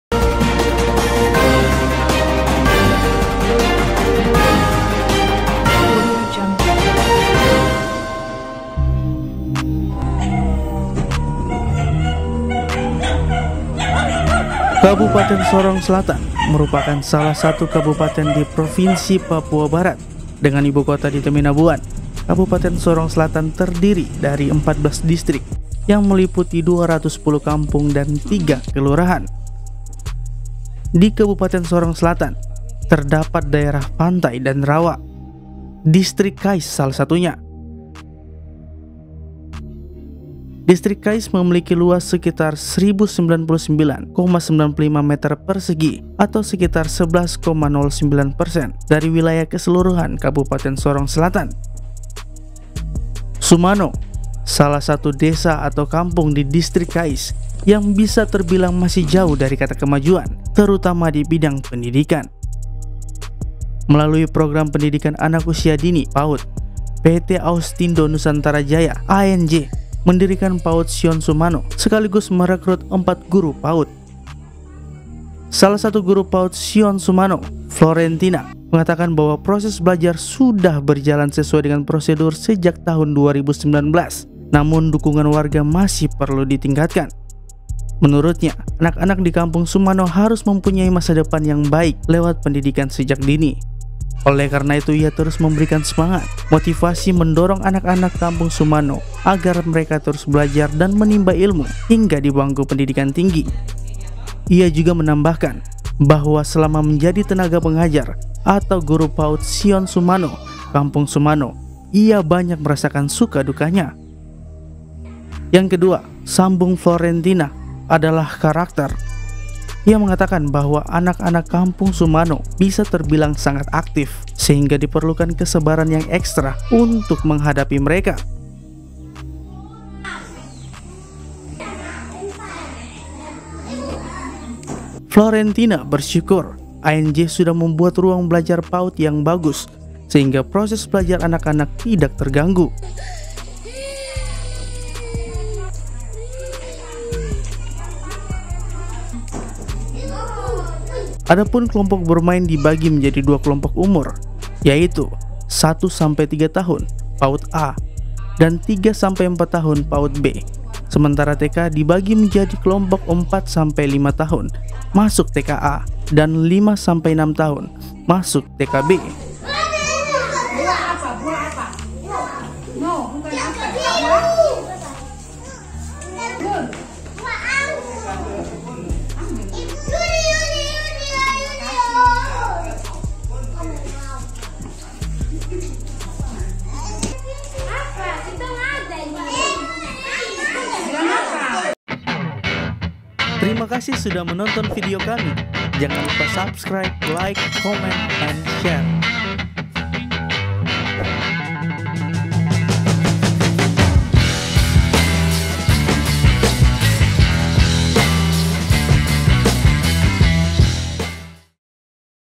PAUD SION SUMANO, MEMBANGUN MASA DEPAN ANAK ANAK PAPUA
TEROPONGNEWS.COM, SORONG SELATAN – Kabupaten Sorong Selatan merupakan salah satu Kabupaten di Provinsi Papua Barat. Dengan ibukota di Teminabuan, Kabupaten Sorong Selatan terdiri dari 14 distrik yang meliputi 210 kampung dan 3 kelurahan.

Di kabupaten Sorong selatan terdapat daerah pantai dan rawa. Distrik Kais salah satunya. Distrik Kais memilik luas  1.099,95 m2 atau sekitar 11,09 persen dari wilayah keseluruhan Kabupaten Sorong Selatan.

Sumano, salah satu desa/kampung di Distrik Kais yang bisa terbilang masih jauh dari kata kemajuan, terutama di bidang pendidikan.

Melalui program Pendidikan Anak Usia Dini (PAUD), PT Austindo Nusantara Jaya (ANJ) mendirikan PAUD Sion Sumano, sekaligus merekrut 4 guru PAUD.

Salah satu Guru PAUD Sion Sumano, Florentina mengatakan bahwa proses belajar sudah berjalan sesuai dengan prosedur sejak tahun 2019. Namun, dukungan warga masih perlu ditingkatkan.

Menurutnya, anak-anak di kampung Sumano harus mempunyai masa depan yang baik lewat pendidikan sejak dini. Oleh karena itu ia terus memberikan semangat, motivasi serta mendorong anak-anak kampung Sumano agar mereka terus belajar dan menimba ilmu hingga di bangku pendidikan tinggi.

Ia juga menambahkan bahwa Selama menjadi tenaga pengajar atau guru PAUD Sion Sumano kampung Sumano,  ia banyak merasakan suka dukanya.

Yang kedua, sambung Florentina, adalah karakter. Ia mengatakan bahwa  anak-anak kampung Sumano bisa terbilang sangat aktif, sehingga diperlukan kesabaran yang ekstra untuk menghadapi mereka.

Florentina bersyukur ANJ sudah membuat ruang belajar PAUD yang bagus sehingga proses belajar anak-anak tidak terganggu.

Adapun Kelompok bermain dibagi menjadi dua kelompok umur, 1-3 tahun (PAUD A) dan 3-4 tahun (PAUD B), sementara TK dibagi menjadi kelompok 4-5 tahun (TK A) dan 5-6 tahun (TK B).